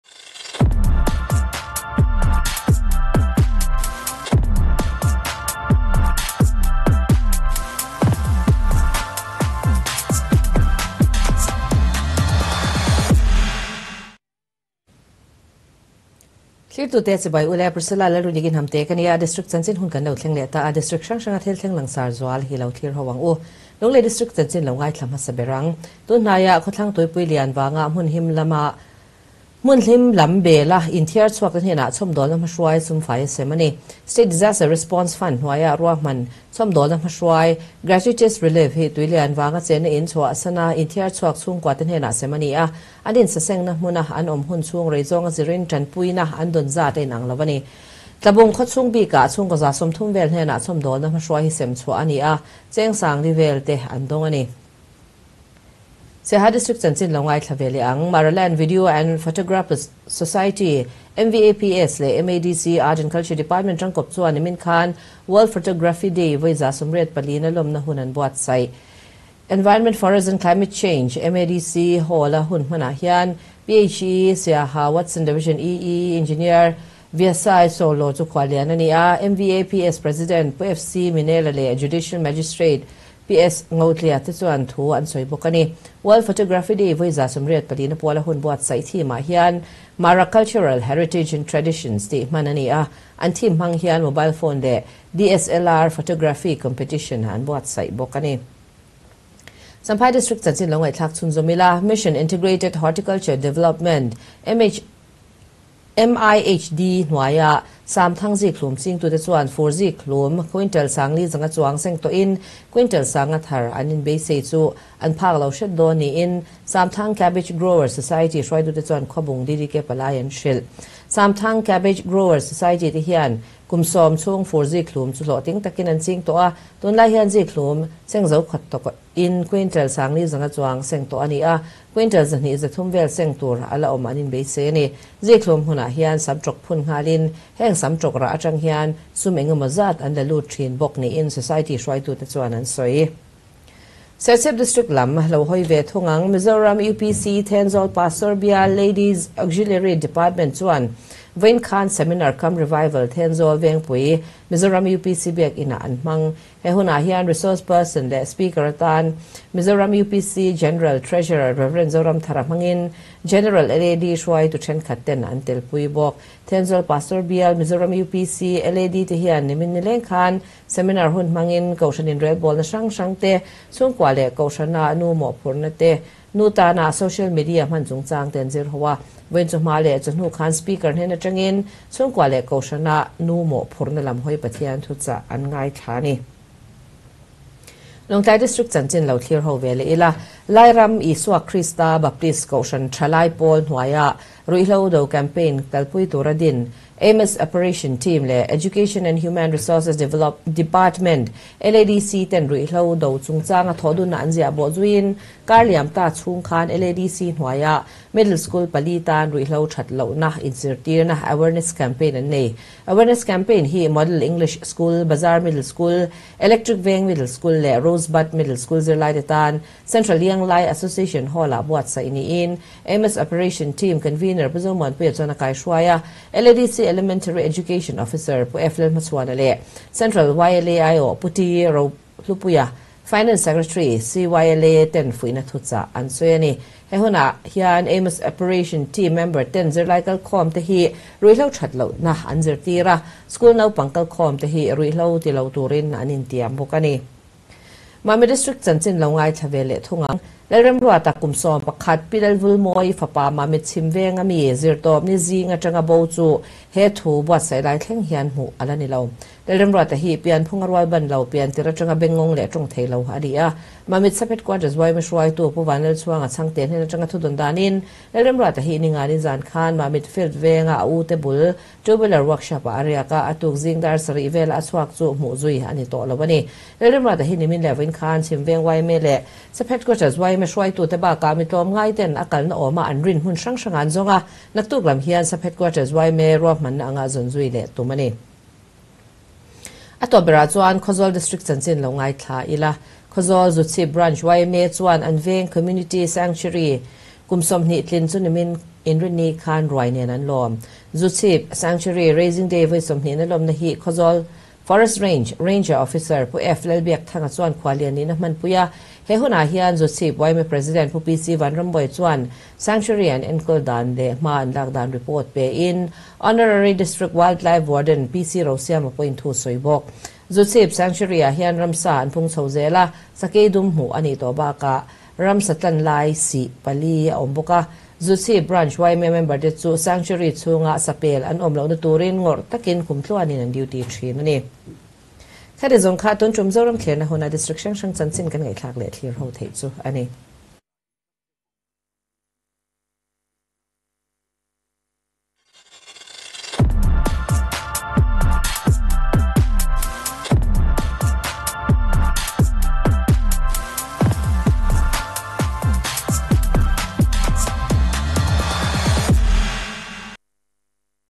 DD News Mizoram - Zoram Thlirna | 20 August 2023 | 5:00 PM
HEADLINES:

1. Lunglei District: Tun hnaia Khawthlangtuipui lian vanga hmun him lam bela inthiarchhuakte hnenah chhawmdawlna hmahruai sumfaia sem a ni.

2. Siaha District: Maraland Video and Photographer's Society (MVAPS) leh MADC Art & Culture Department tangkawp chuan nimin khan World Photography Day vawi 184-na lawmna hun an buatsaih.

3. Champhai District: Mission Integrated Horticulture Development (MIHD) hnuaia Samthang Zikhlum chingtute chuan fur zikhlum (Ryozeki) quintal 4500 chuang seng tawhin quintal 5000 thar an inbeisei chu an phak lo hret dawn niin Samthang Cabbage Grower Society hruaitute chuan Khawbung DDK palai an hrilh.

4. Serchhip District: Mizoram UPC, Thenzawl Pastor bial Ladies Auxiliary Department chuan vawiin hian Seminar cum Revival, Thenzawl Vengpui Mizoram UPC Biak Inah an hmang.

5. Lawngtlai District: Lairam Isua Krista Baptist Kohhran Thalai Pawl hnuaia Ruihhlo do campaign kalpui tura din Amos Operation Team leh Education & Human Resources Department, LADC ten ruihhlo do chungchanga thawhdunna an ziah bawhzuiin kar liam ta chhung khan khan LADC hnuaia Middle School pali tan Ruihhlo that lohna inzirtirna (Awareness Campaign) an nei.

6. Mamit District: Lalremruata kum 11, Pi Lalvulmawii fapa Mamit Chhimveng a mi, Zirtawpni zing atanga bo chu he thu buatsaih lai thleng khan hmuh a la ni lo.

7. Khawzawl District: Khawzawl Zuchhip Branch YMA chuan an veng Community Sanctuary kum 20 tlin chu nimin Inrinni khan ruai nen an lawm.